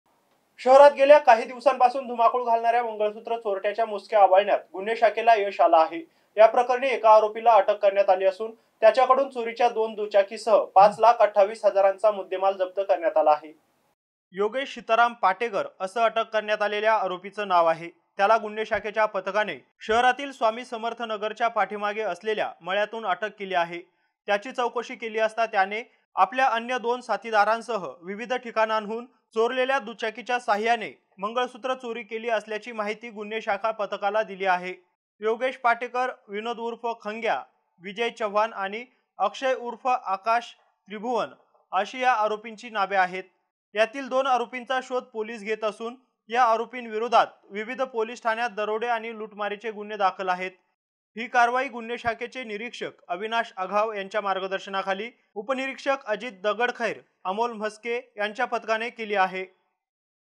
MCN NEWS| मंगळसुत्र चोरीचा धुमाकूळ घालणारे आरोपी गुन्हे शाखेच्या ताब्यात
छत्रपती संभाजीनगर शहरात गेल्या काही दिवसांपासून धुमाकूळ घालणाऱ्या मंगळसूत्र चोरट्याच्या मुसक्या आवळण्यात गुन्हे शाखेला यश आलय. याप्रकरणी एका आरोपीला अटक करण्यात आली असून त्याच्याकडून चोरीच्या 2 दुचाकीसह 5 लाख 28 हजारांचा मुद्देमाल जप्त करण्यात आलाय.